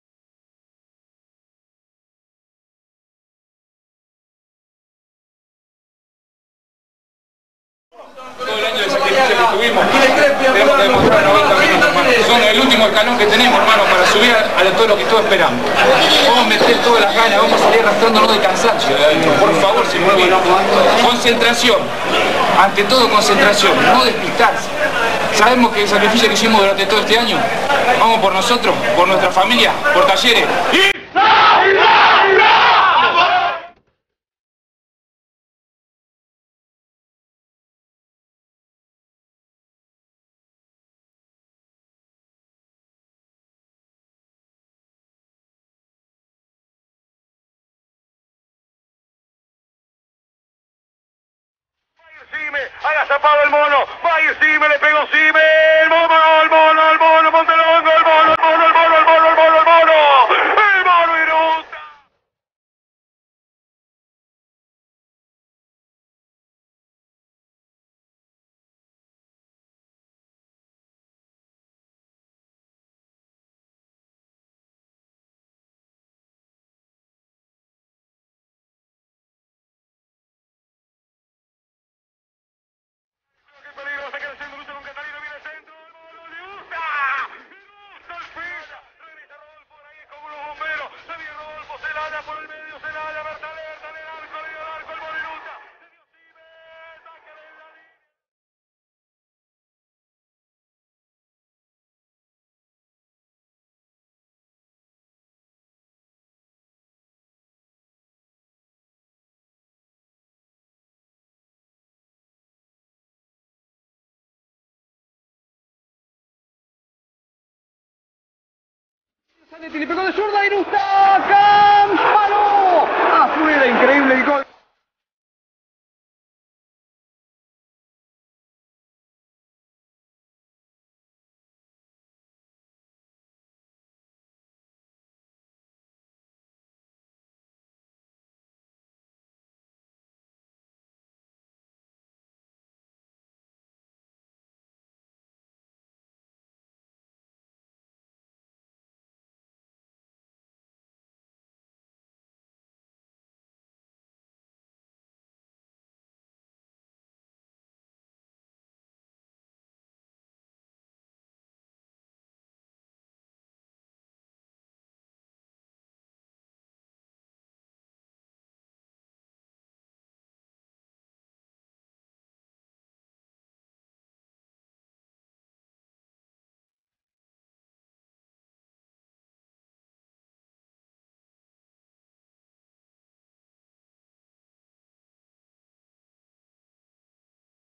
Gustavo Irusta: A 23 años de su debut en Talleres
Nuestro reconocimiento a uno de los grandes arqueros del Club.